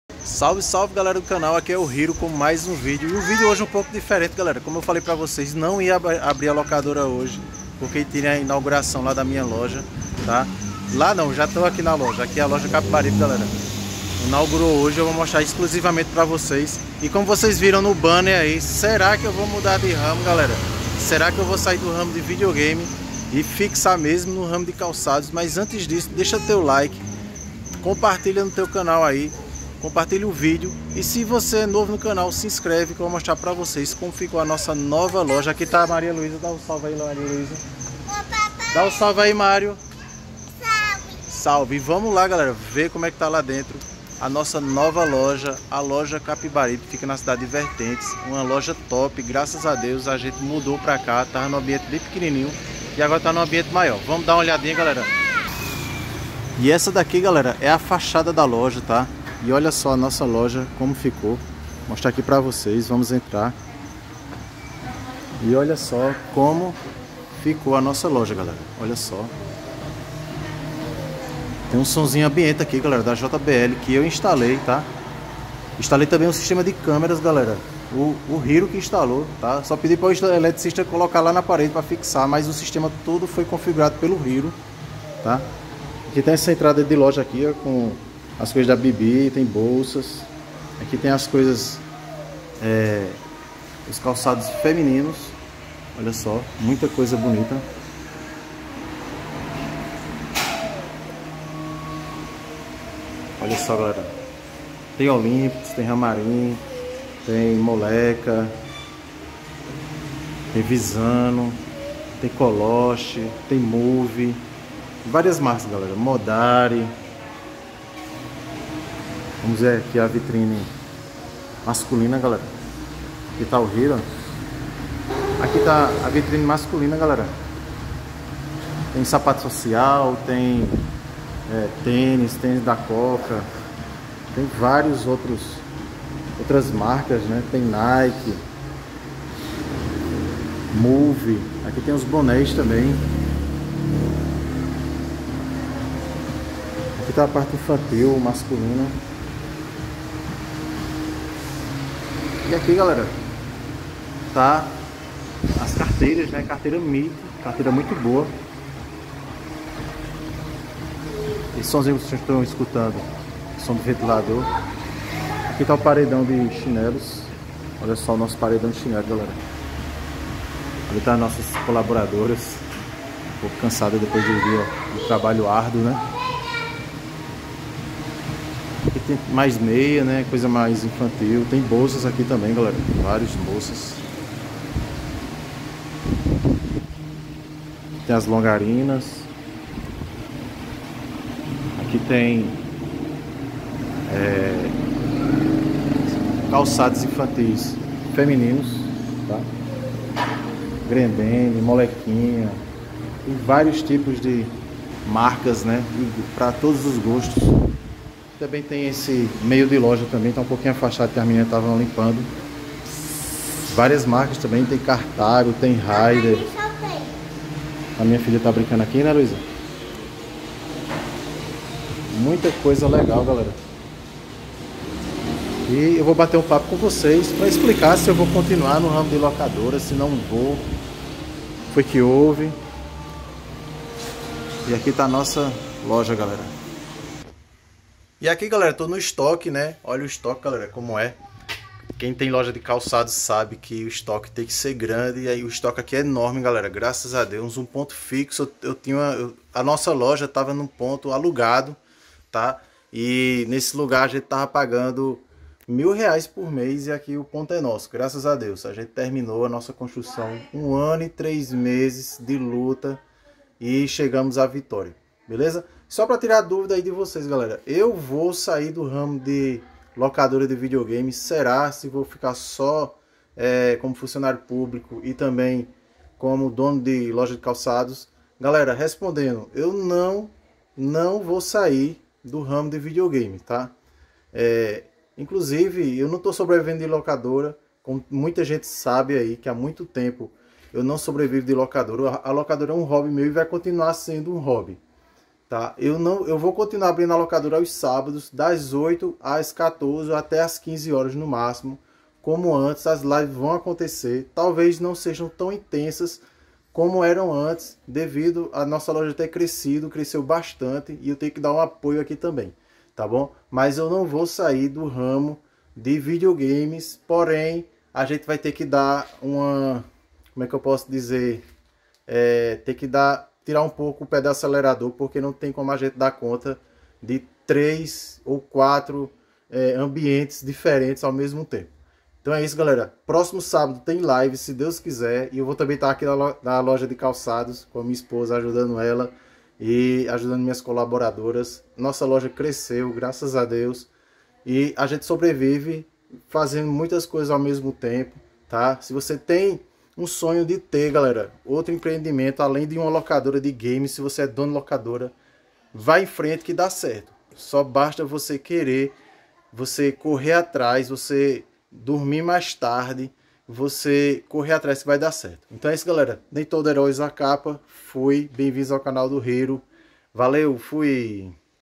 mudei de ramo? e a locadora como fica? vai ter live ainda?
veja nossa nova loja que inauguramos e se ainda vai ter live direto da locadora de games.